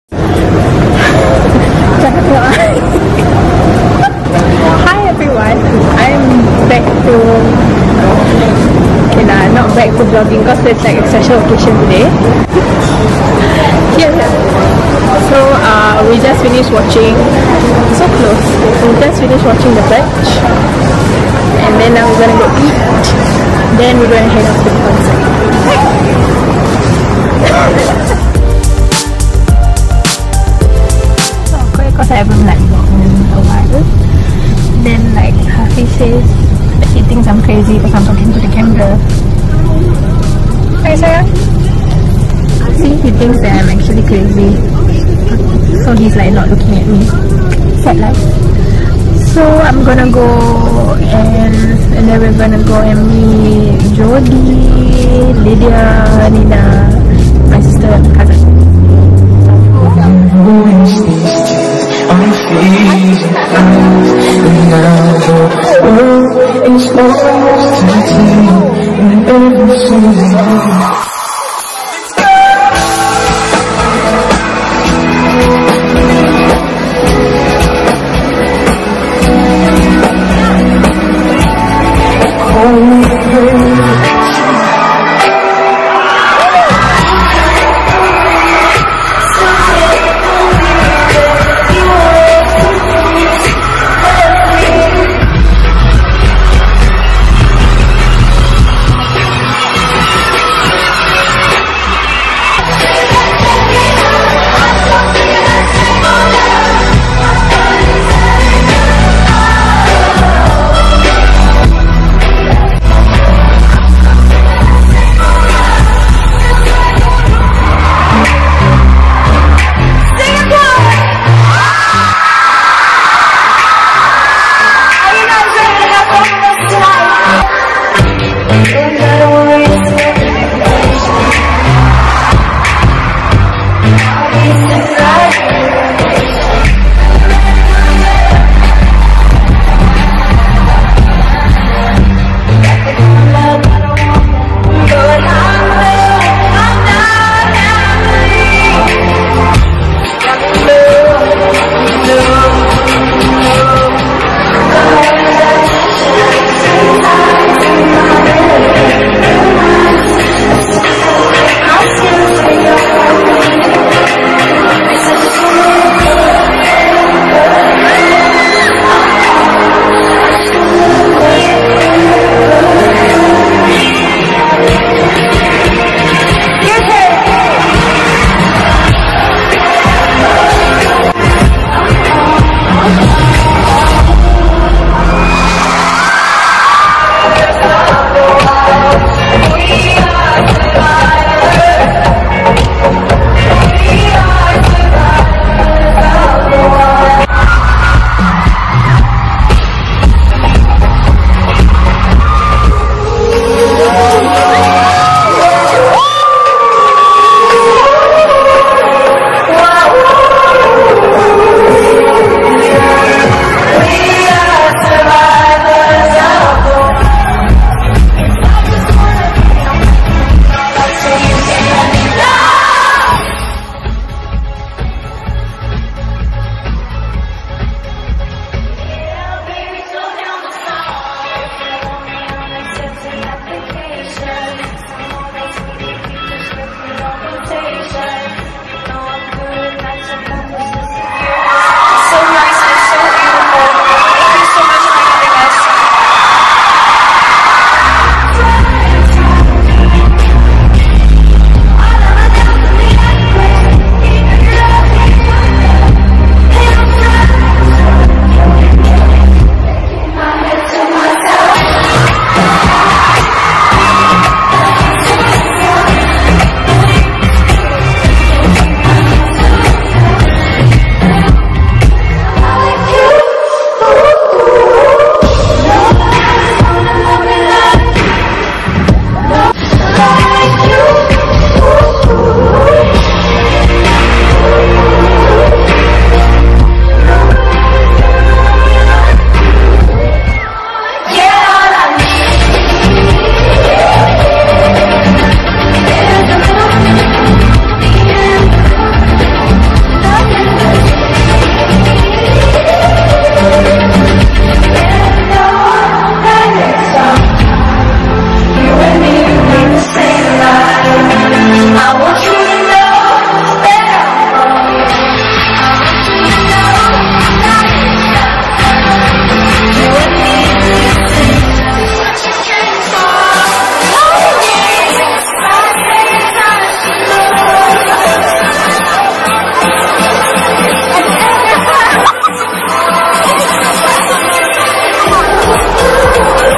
Revival Tour Singapore Feat. Gentle Bones
Went to catch a movie & had a quick dinner before heading over to watch Selena's concert. It was so amazing! I had a wonderful time!